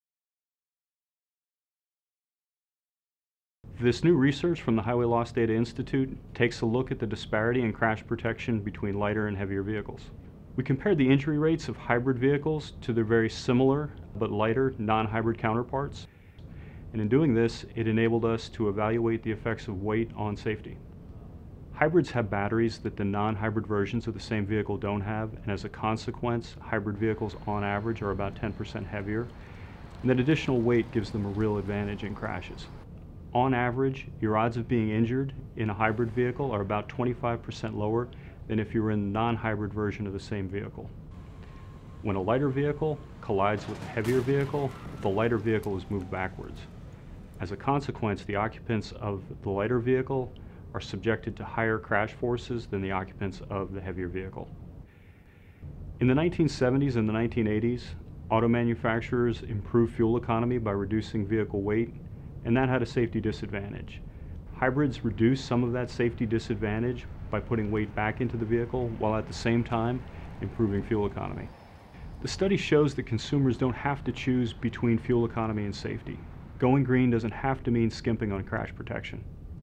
Hybrid models have lower injury risk
IIHS news release • November 17, 2011

Hybrid models have lower injury odds than their conventional counterparts

Hybrids have a safety edge over their conventional twins when it comes to shielding their occupants from injuries in crashes, new research by the Highway Loss Data Institute (HLDI), an affiliate of the Insurance Institute for Highway Safety, shows. On average, the odds of being injured in a crash are 25 percent lower for people in hybrids than people traveling in nonhybrid models.